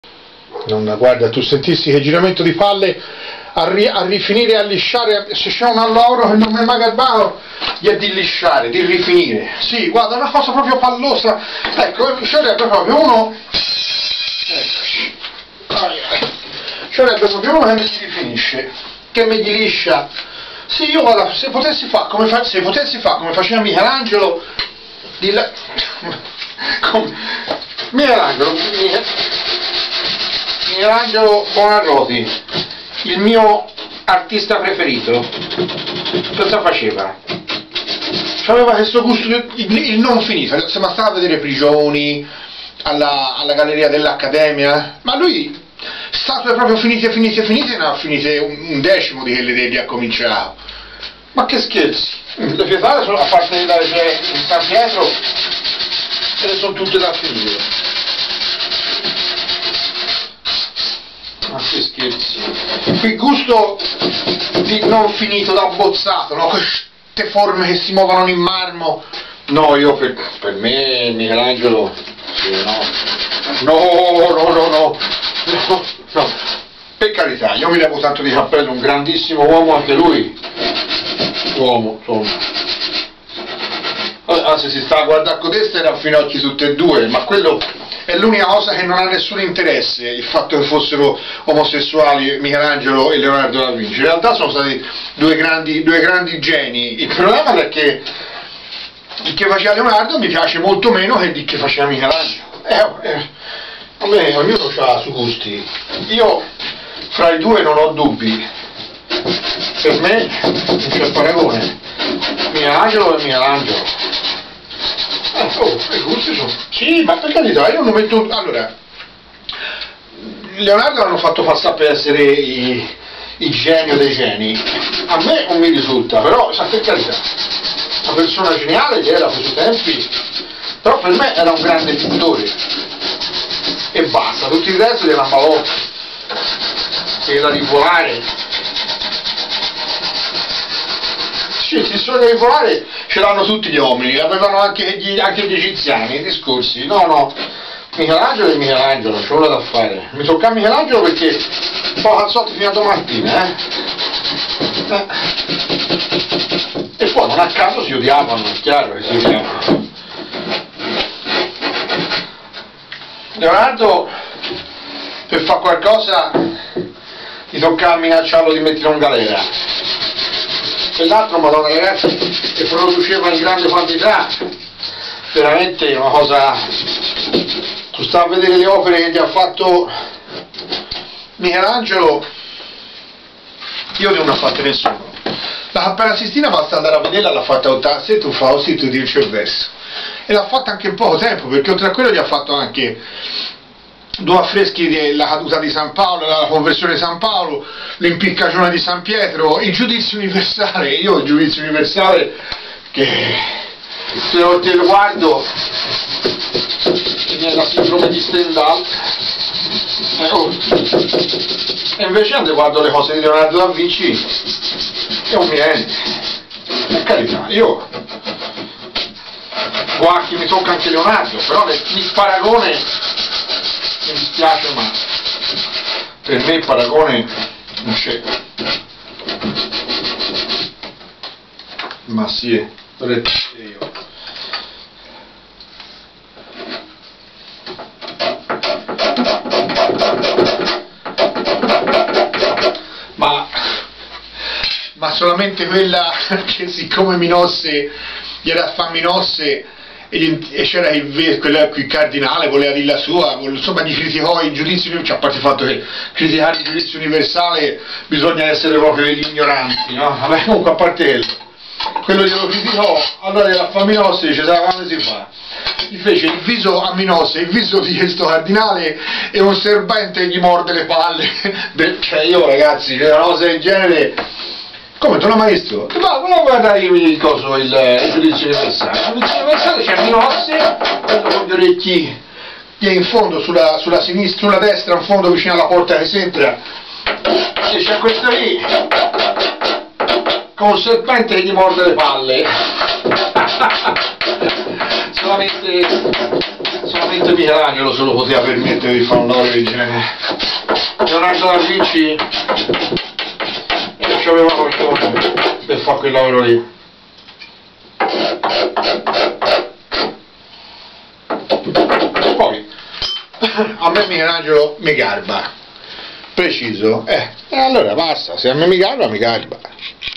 Oh bischero! Ma vuoi mettere Michelangelo con Leonardo?
La viola da gamba comincia già a camminare da sola, tra poco canterà!
Il M°Fabio Chiari liutaio non tralascia di esprimere il suo giudizio sui non finiti del suo grande artista preferito, Michelagnolo Buonarroto. Ovviamente l'allievo Fellini non può non portare a paragone il grande antagonista di Michelangelo, messer Lionardo da Vinci. Al che il M° si infervora e parte per una filippica a sostegno della sua tesi di base: Non vorrai mica mettere Michelangelo con quel bischero di Leonardo? E qui si apre la discussione.